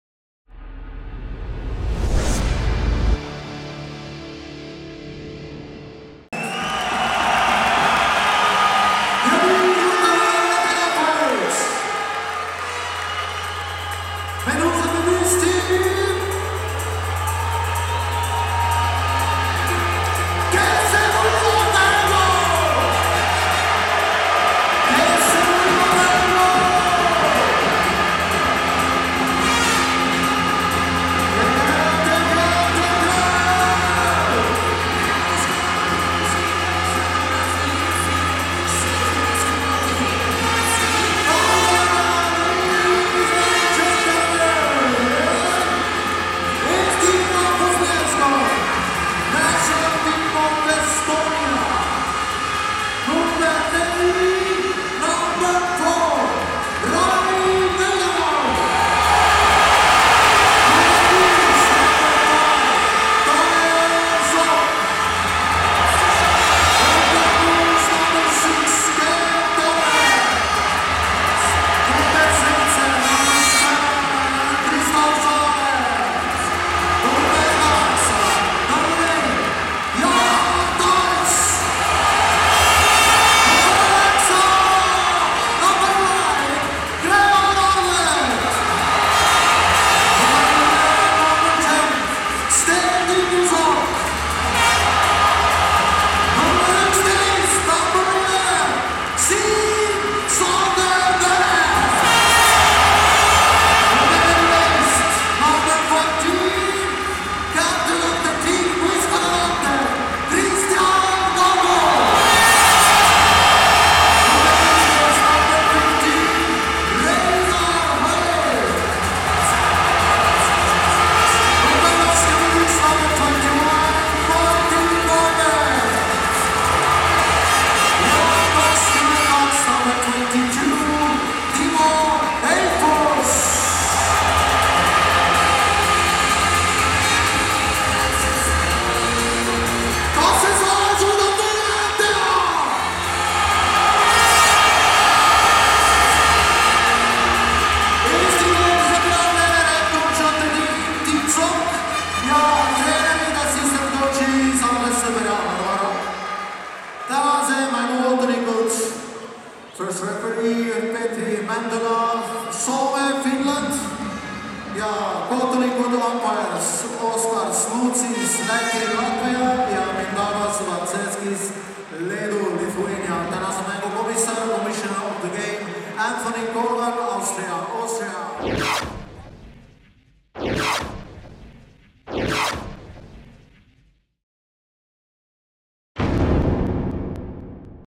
2013 Tutvustus
Eurobasket 2015 kvalifikatsiooni poolfinaal Eesti - Bulgaaria 6500 pealine publik tervitamas oma meeskonda.